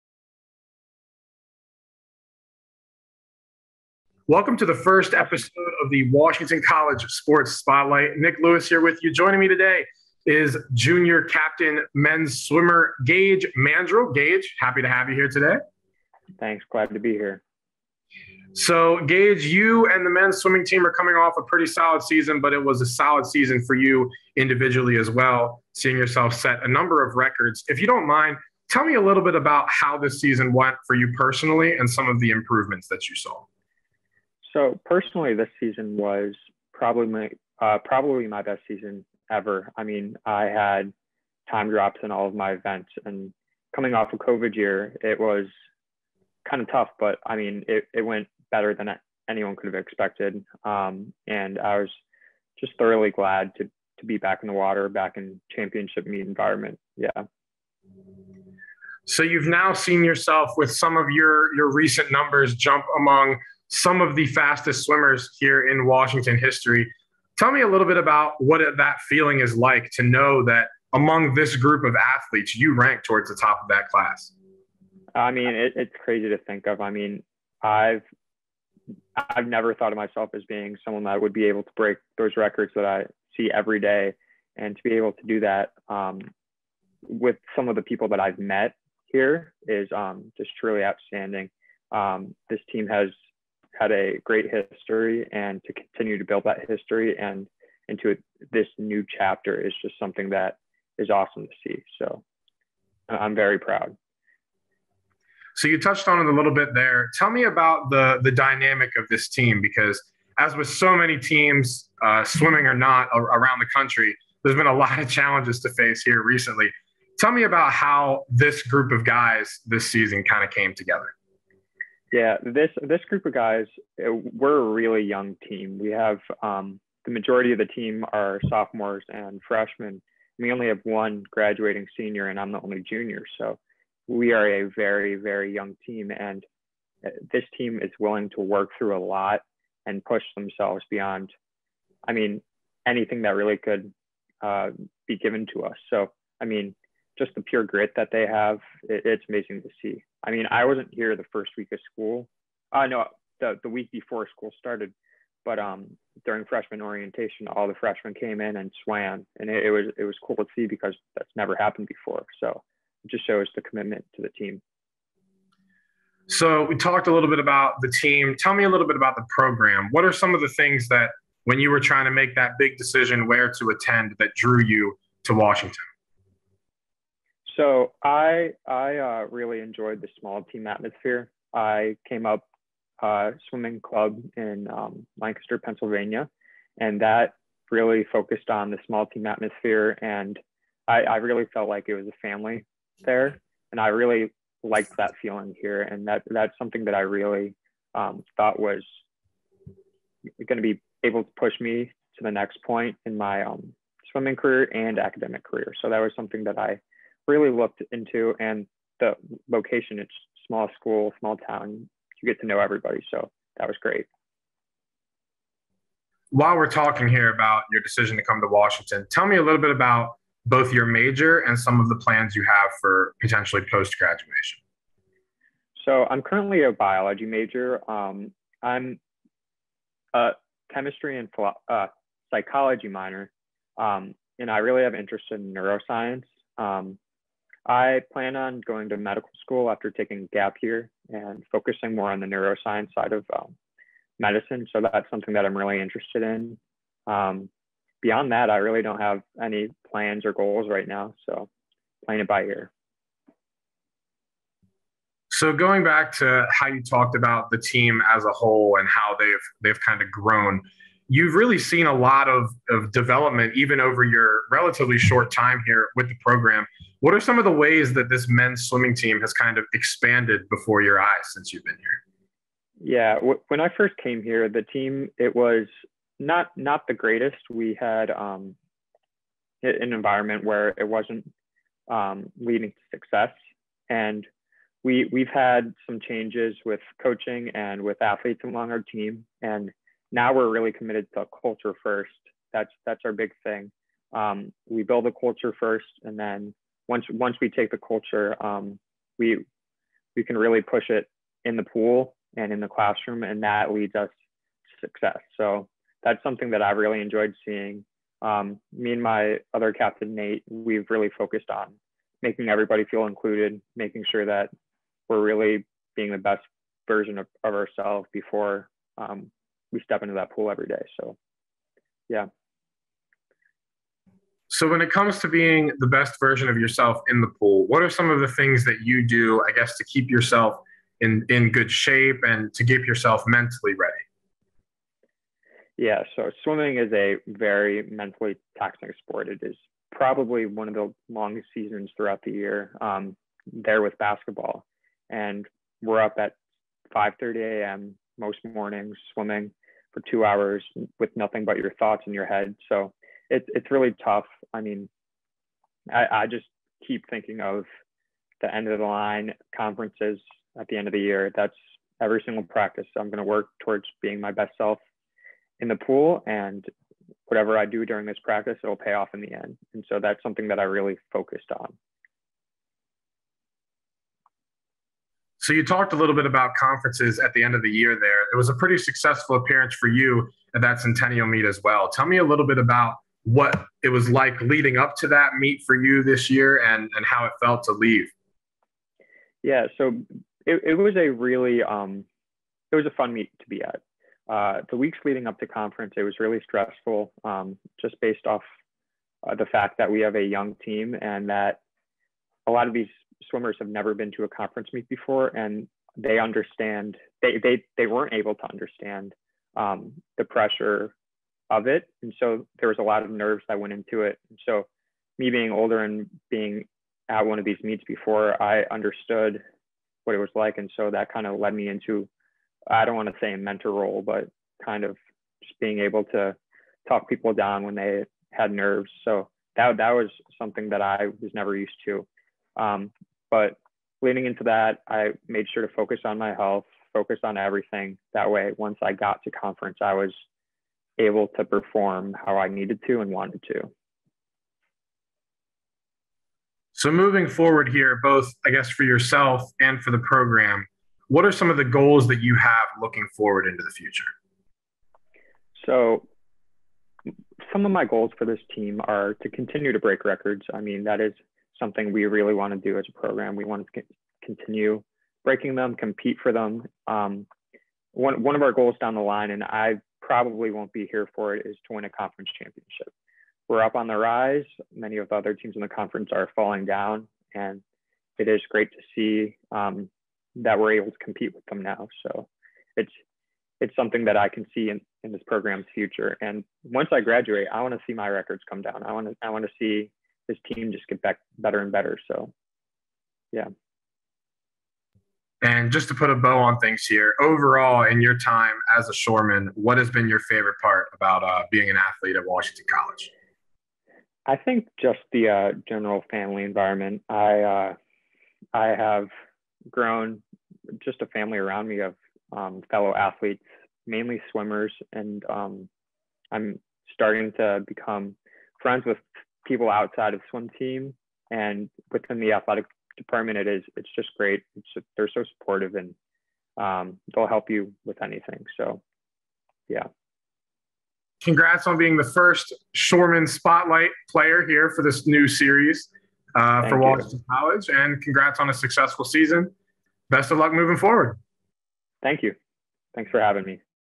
Washington College Sports Spotlight, Gage Mandrell (Men's Swimming)
The spotlight shines on record-breaking junior swimmer Gage Mandrell.